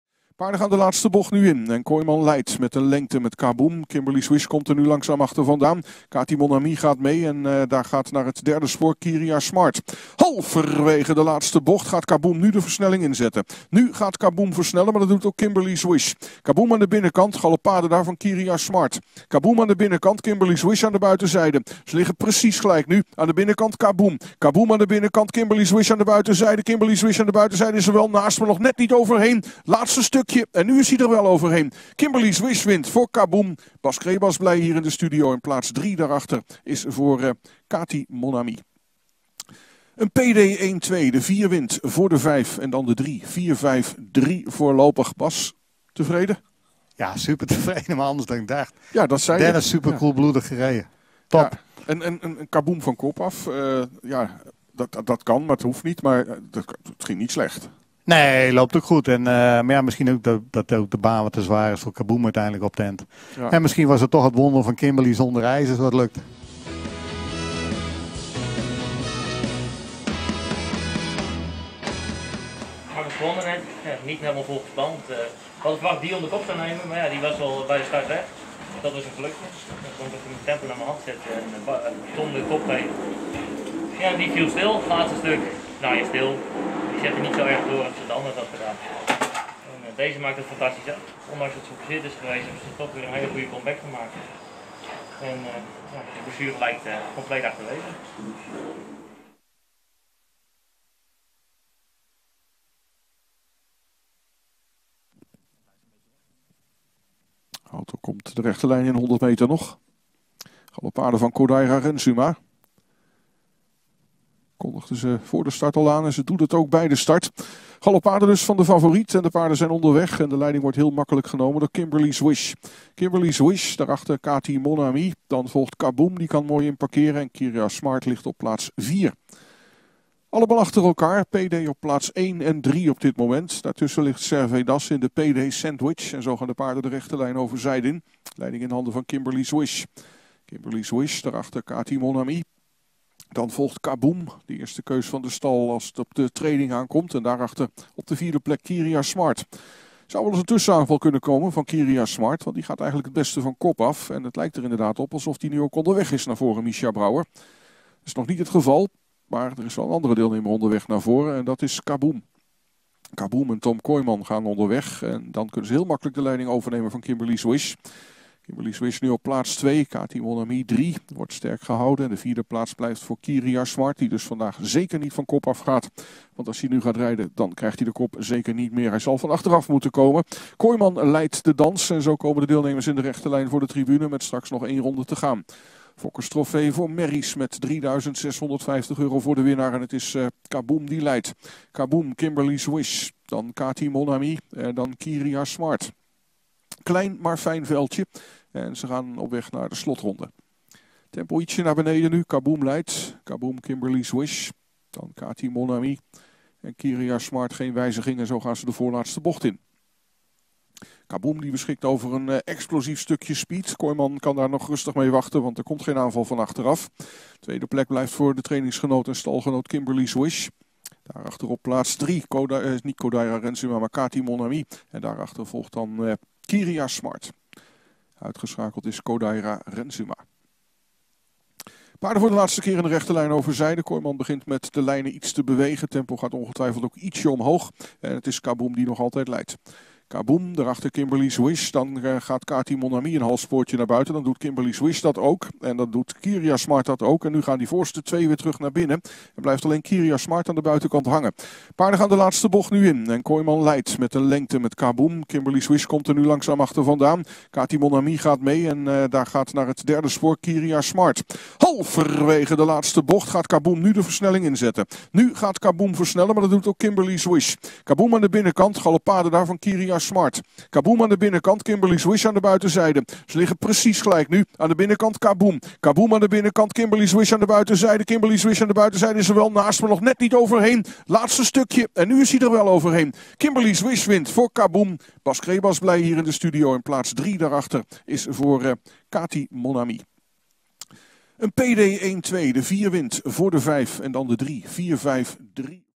Kimberly's Wish & Dennis Minnema winnen Fokkers Trofee K Merries (NL) - Mons 4 dec. 2020
Kimberly's Wish & Dennis Minnema winnen Fokkers Trofee K Merries (NL) voor Payday Racing en tr. Bas Crebas - Mons 4 dec. 2020
E.: Stal Payday - Peter Delis - F. Fam. H. Kloosterman
F. Fam. H. Kloosterman